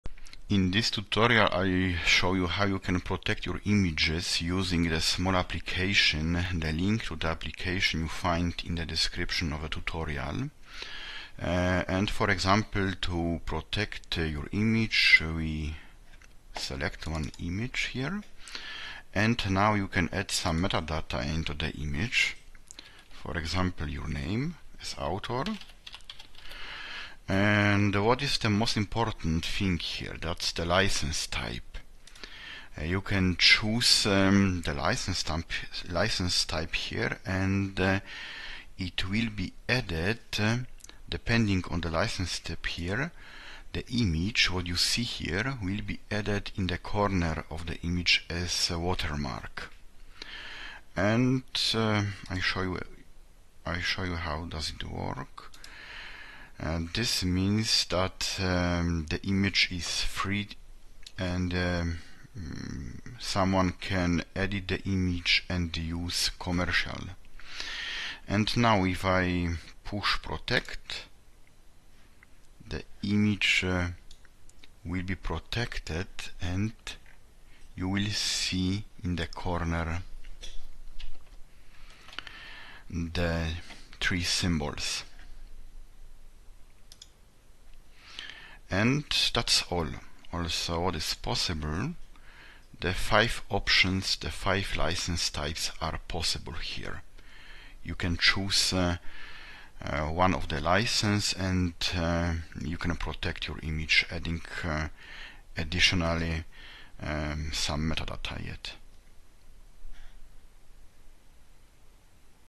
How To Protect Photos Using The Small Application. Link To Download In Description
Protecting your photos with watermarks is crucial in today's digital age. Learn how to safeguard your precious photos and maintain control over their distribution with effective watermarking techniques. Explore the importance of watermarking and discover easy-to-use tools and strategies to add watermarks seamlessly to your images. Don't let your creativity be compromised – defend your photos with watermarks today.